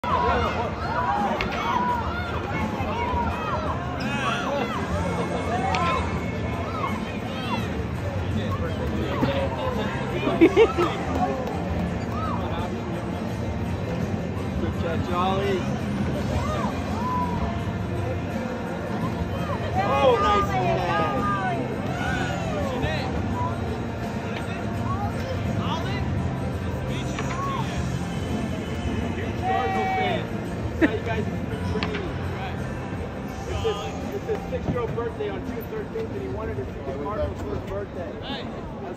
5 year old plays catch with St Louis Cardinal pitcher TJ McFarland during batting practice. MLB
When asked what he wanted for his 6 year old birthday. Ollie Feld told his parents he wanted to see Albert Pujols and the St Louis Cardinals play.  June 8th 2022 Ollie attended the St Louis Cardinal game vs Tampa Bay Rays. During batting practice Ollie was hanging out in the outfield seats hoping to catch a home run or a toss up from one of the St Louis Cardinals. Thanks to TJ McFardland a relief pitcher on the St Louis Cardinals, Ollie got an opportunity that every kid dreams of. TJ not only threw Ollie a ball he also took the time to play catch with Ollie. For the rest of the trip all Ollie could talk about was that he played catch with a major league pitcher. As a 5 year old, playing catch with a major league player is an opportunity of a life time. With players like TJ McFardland, it’s no secret why the St Louis Cardinals have such a strong fan base.  TJ taking the time to play catch with Ollie was a class act. Thank you for your time TJ You are a class act.  Ollie now owns three jerseys Albert Pujols, Babe Ruth and TJ McFarland. His three favorite players.